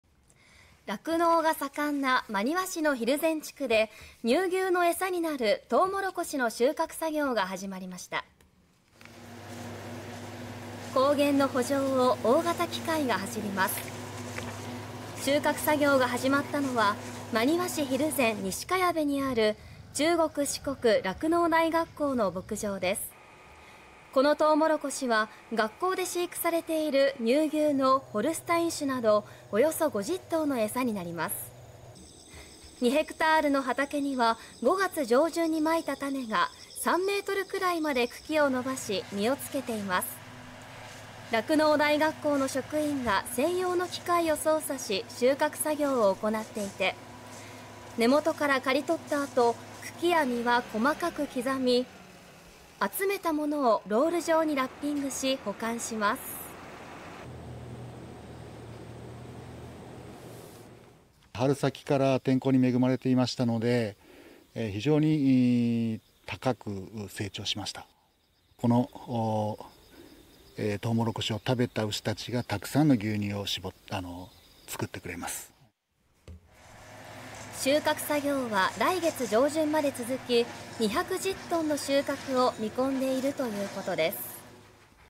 乳牛のエサに…酪農大学校でトウモロコシの収穫始まる １０月上旬まで【岡山・真庭市】 (22/09/13 16:00)
酪農が盛んな真庭市の蒜山地区で乳牛の餌になるトウモロコシの収穫作業が始まりました。

高原の「ほ場」を大型機械が走ります。収穫作業が始まったのは真庭市蒜山西茅部にある中国四国酪農大学校の牧場です。

このトウモロコシは、学校で飼育されている乳牛のホルスタイン種など約５０頭の餌になります。

２ヘクタールの畑には、５月上旬にまいた種が３メートルくらいまで茎を伸ばし、実を付けています。

酪農大学校の職員が専用の機械を操作し収穫作業を行っていて、根元から刈り取ったあと茎や実は細かく刻み、集めたものをロール状にラッピングし保管します。

（中国四国酪農大学校　関哲生第一牧場長）
「春先から天候に恵まれ非常に高く成長した。このトウモロコシを食べた牛たちがたくさんの乳牛を作ってくれる」

収穫作業は１０月上旬まで続き、２１０トンの収穫を見込んでいるということです。